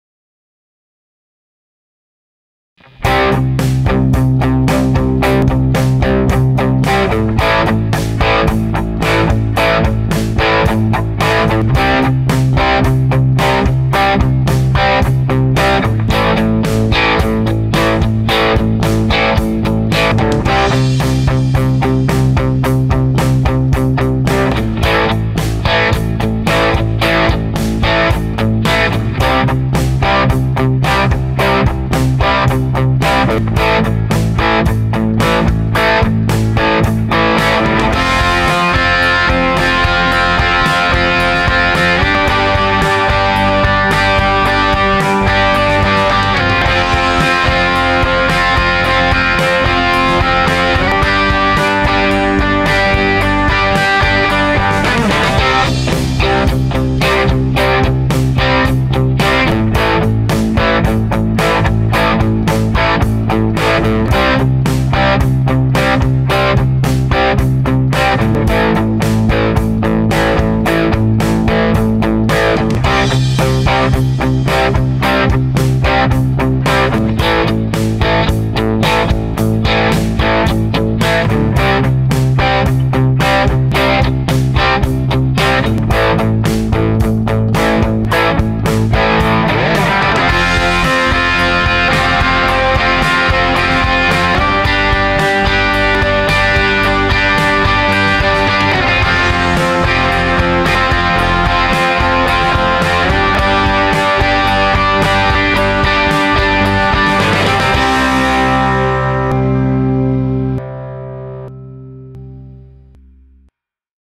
6 Guitarras Diferentes
Comparativo entre 6 diferentes na mesma música.

1 - Epiphone Lucille
2 - Fender Stratocaster American Deluxe
3 - Music Man Axis Supersport
4 - ESP Telecaster Ron Wood
5 - Gibson Les Paul Studio GEM
6 - Fender Stratocaster Eric Johnson